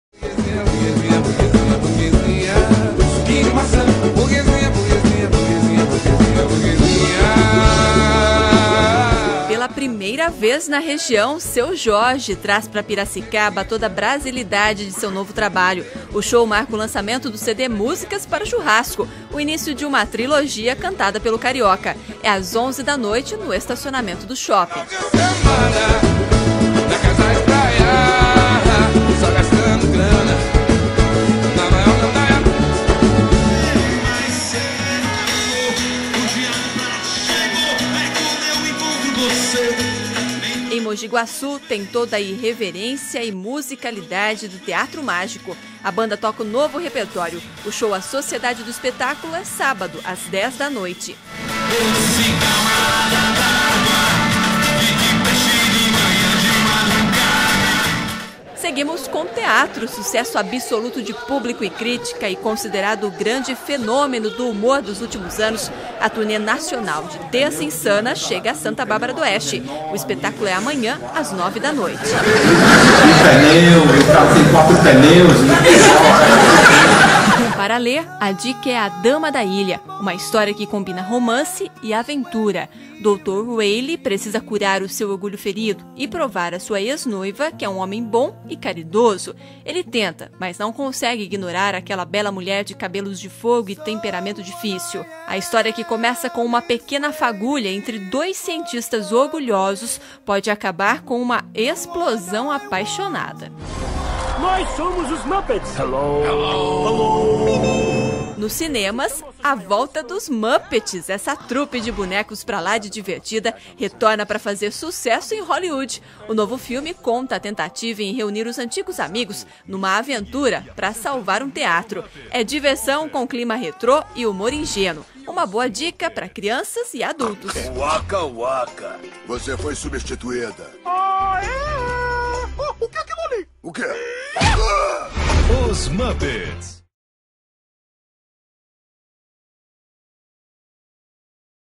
Fim de semana tem Seu Jorge em Piracicaba
Cantor carioca lança novo trabalho em show realizado neste sábado (03). Veja quais as melhores opções de lazer nesse fim de semana na região de Rio Claro.